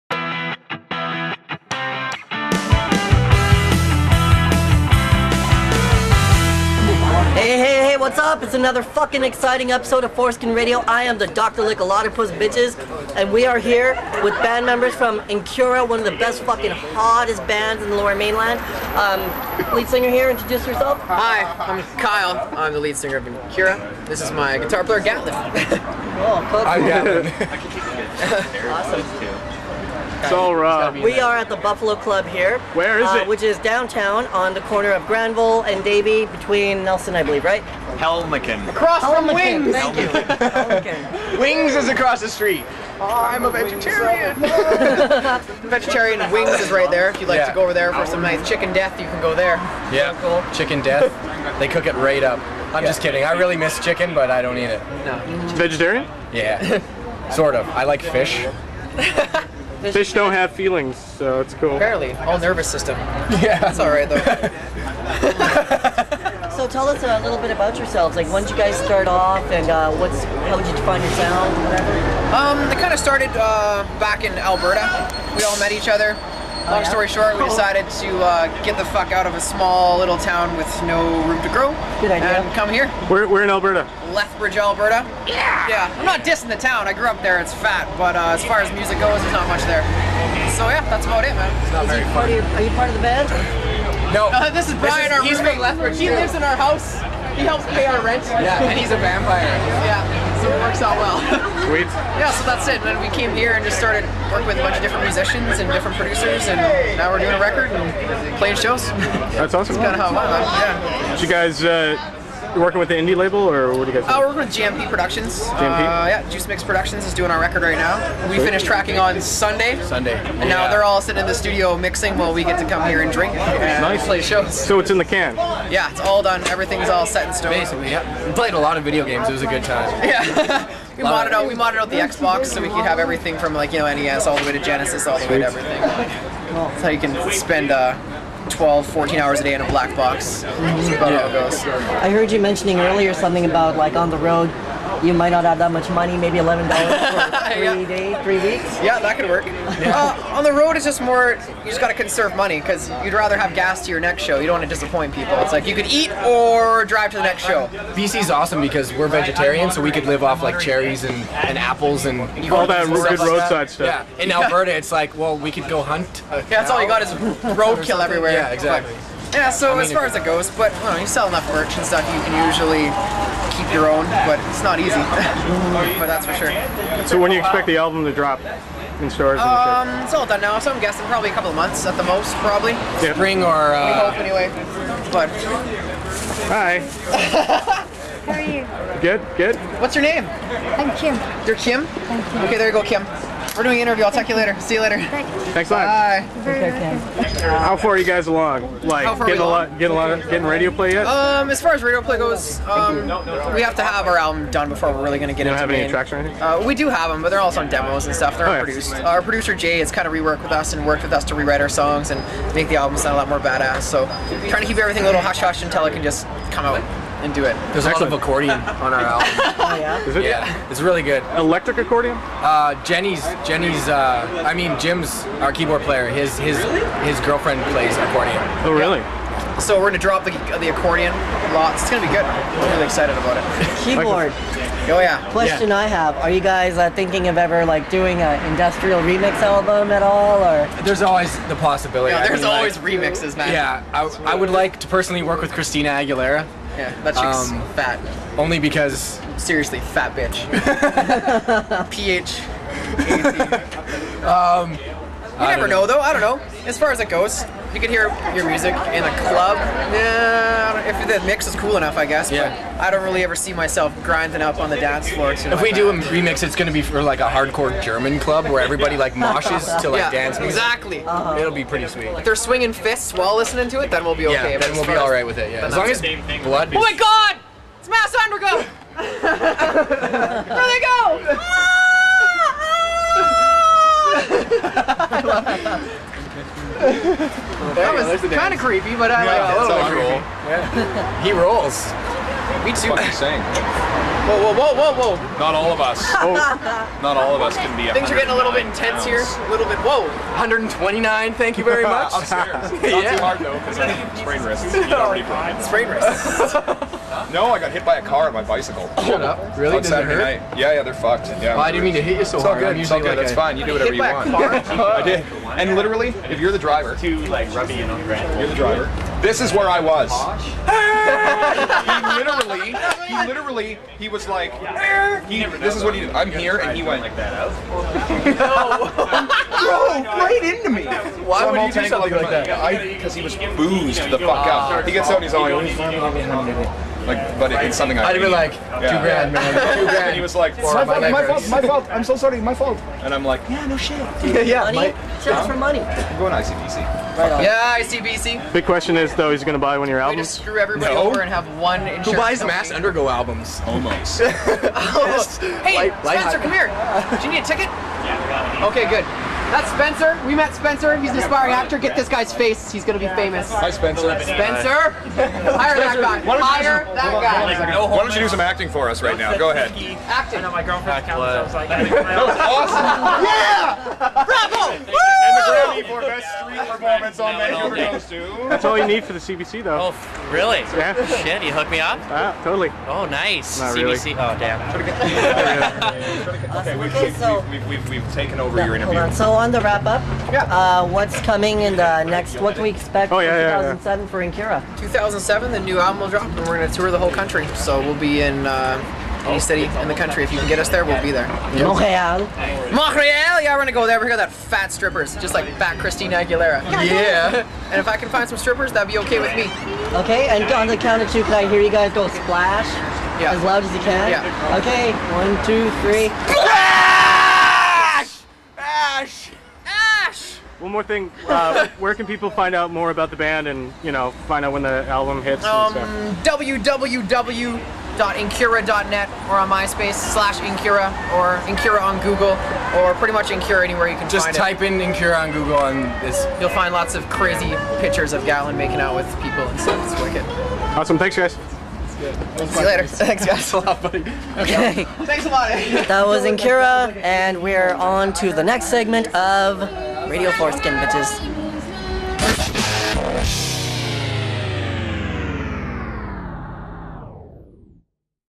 Incura Interview
ForeskinRadio.com interviews rock band Incura outside the Buffalo Club in Vancouver, Canada, February 15, 2007